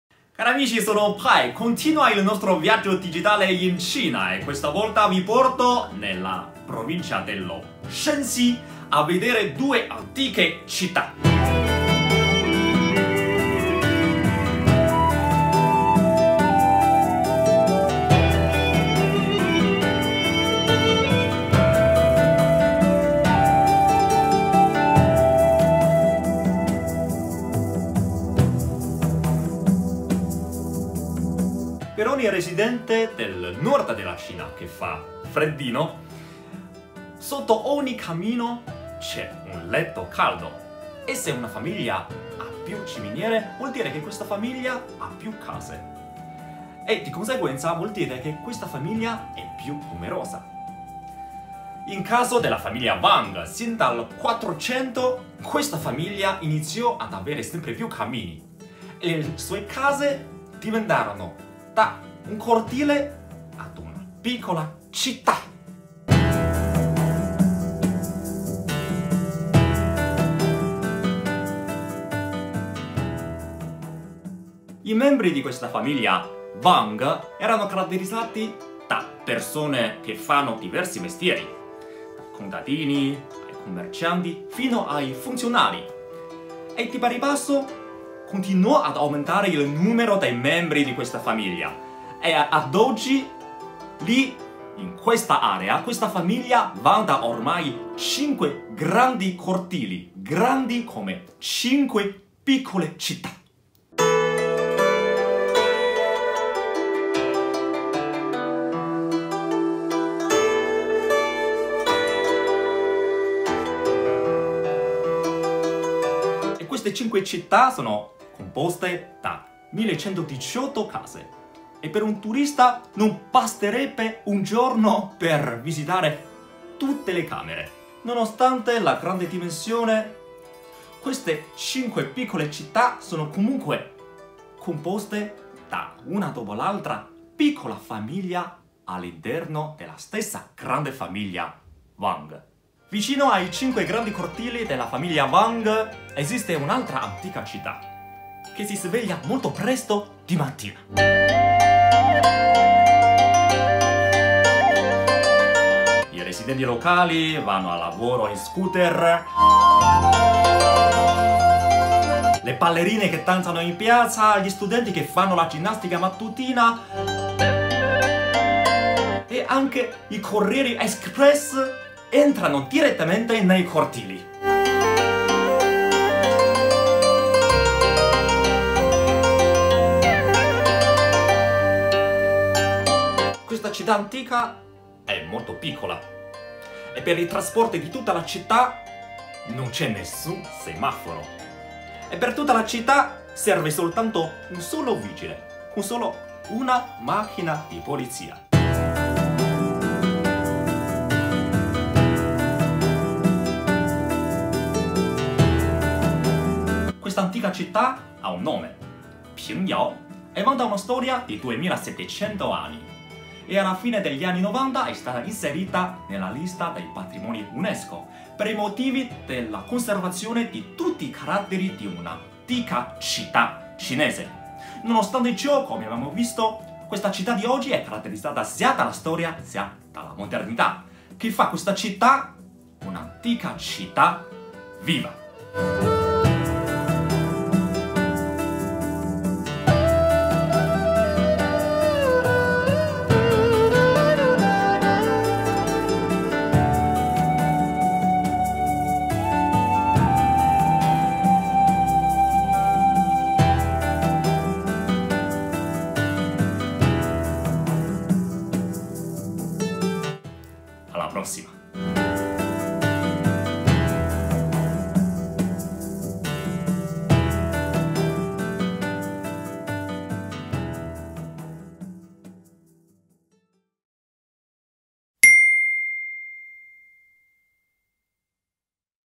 Viaggio digitale in Cina-Pai vi porta alla Provincia dello Shanxi
Viaggio digitale in Cina-Oggi ci dirigiamo con Pai nel nord della Cina, nello Shanxi, per conoscere l'antica famiglia Wang e la tradizionale città di Pingyao, patrimonio dell'UNESCO